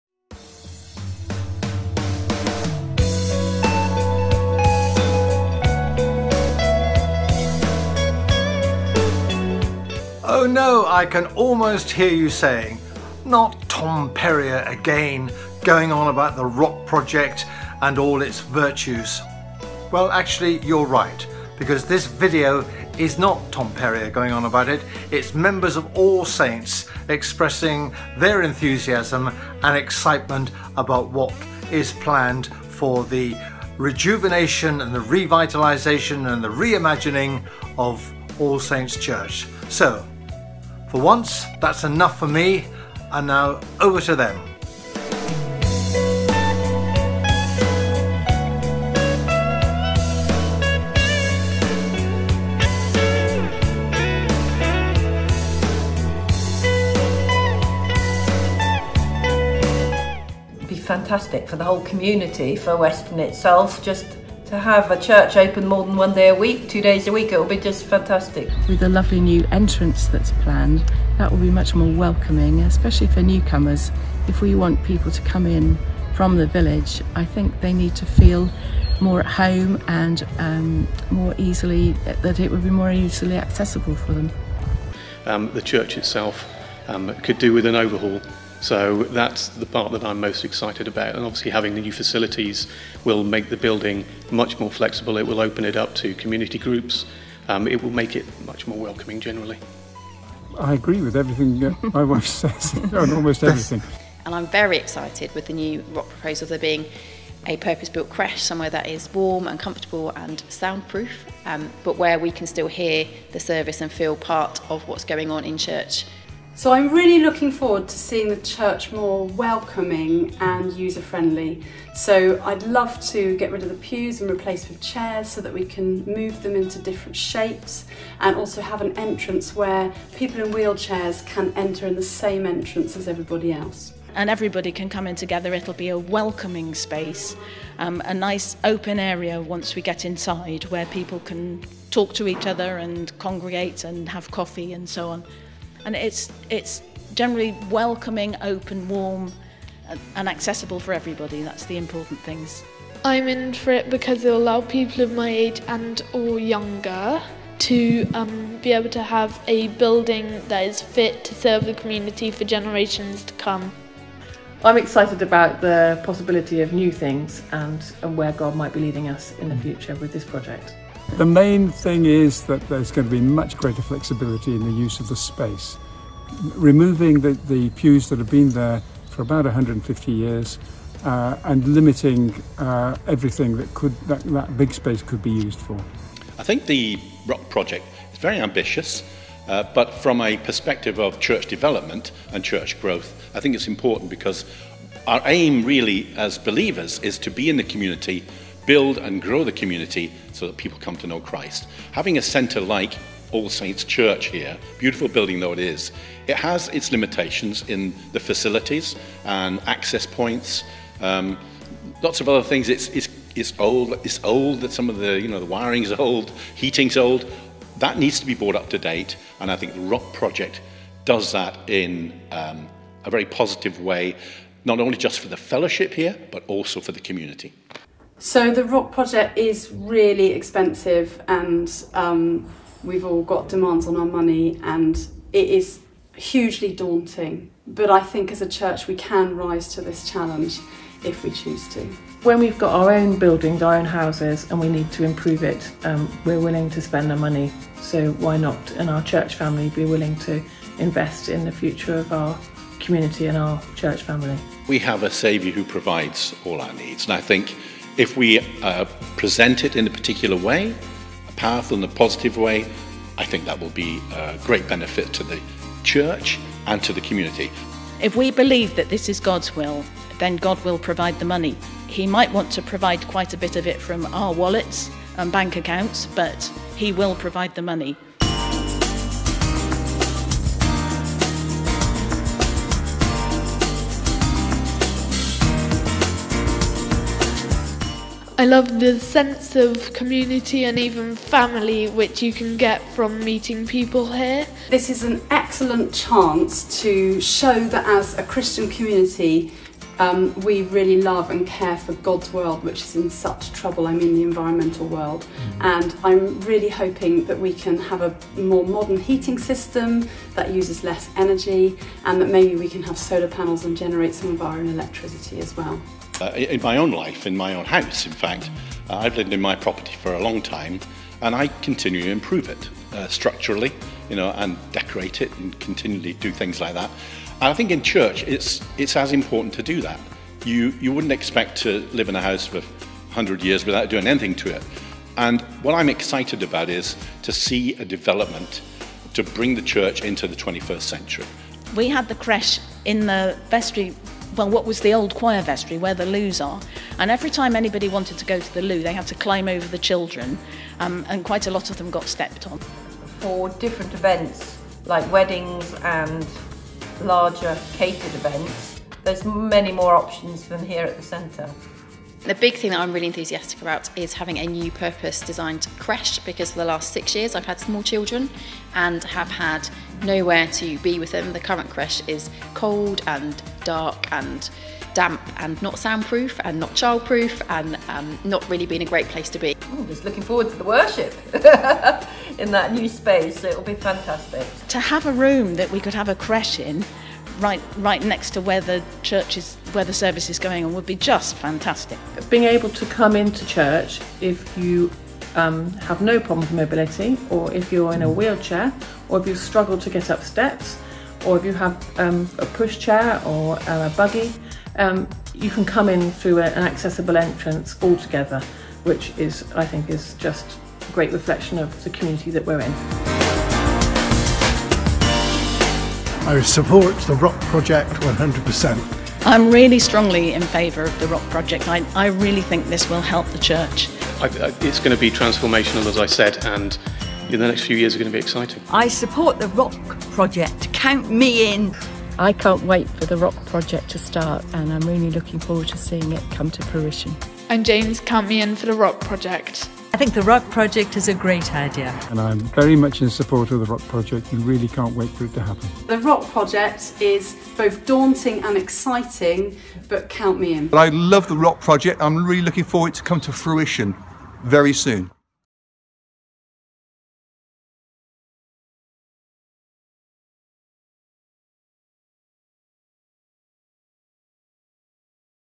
ROCK Project One
On 30 January 2022 we launched our latest ROCK appeal at our Sanctuary and Engage services. Tom Peryer spoke on ‘A church which is wholehearted and committed’.
If you missed the ROCK video that show a number of church members sharing their hopes for the ROCK Project and why they are strongly committed to it, you can view it here.